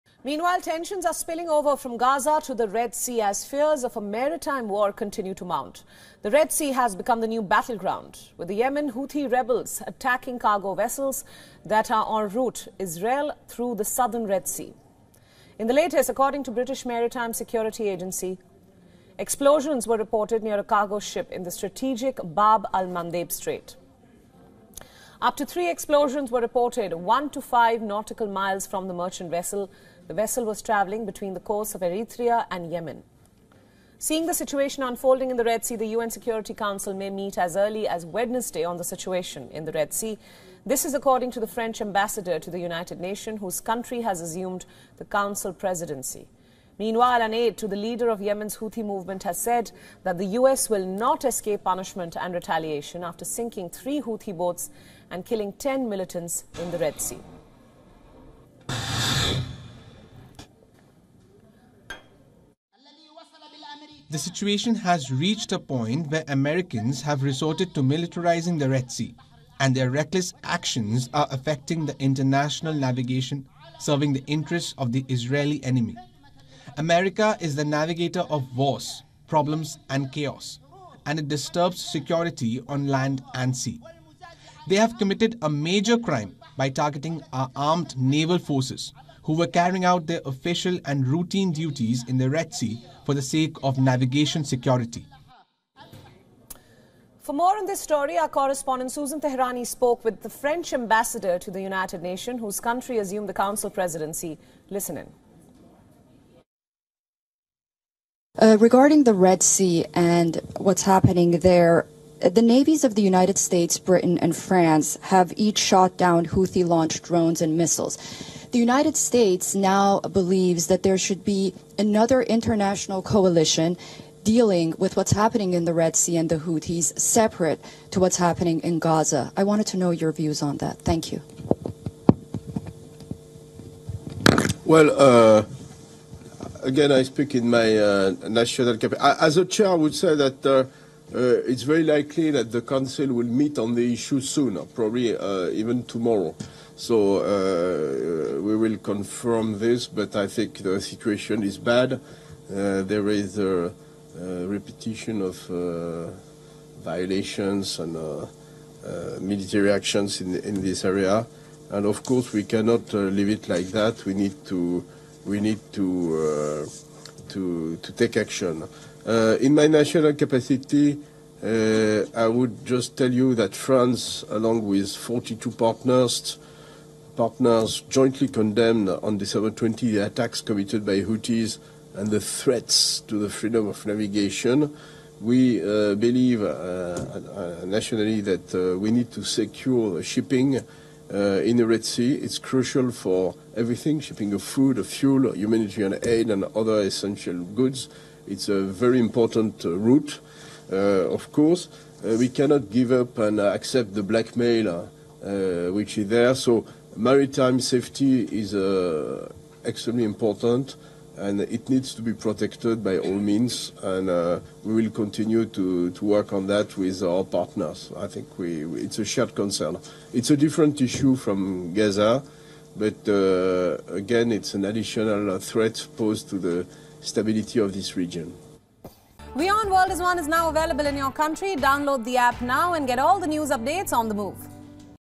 Red Sea Attacks: Up to three explosions reported near cargo ship in Bab-Al-Mandab Strait | WION
As the Red Sea attacks continue, Explosions were reported near Cargo ship in Bab-Al-Mandab strait. The United Nations Security Council will meet to discuss the Red Sea situation. France has assumed the presidency of UN Security Council. Watch to get more insights!